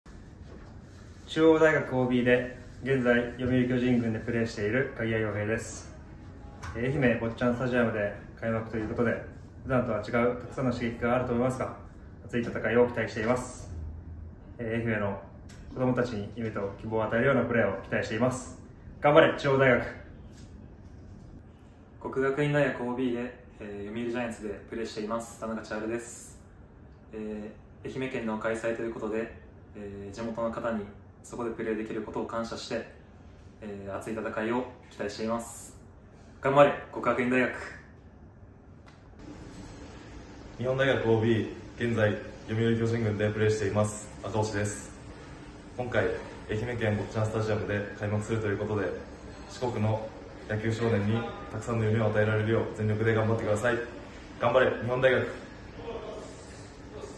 【東都】読売ジャイアンツの選手から激励メッセージ！9/2(土)東都大学野球秋季リーグ戦開幕
スポーツブルでは東都大学野球秋季リーグ戦全試合をライブ配信！
東都大学野球秋季リーグ戦を前に、プロの世界で戦う
鍵谷陽平選手(中央大学OB)
田中千晴選手(國學院大學OB)
赤星優志選手(日本大学OB)
から現役選手へメッセージ！
✅東都大学野球連盟 PREMIUM UNIVERSITIES 22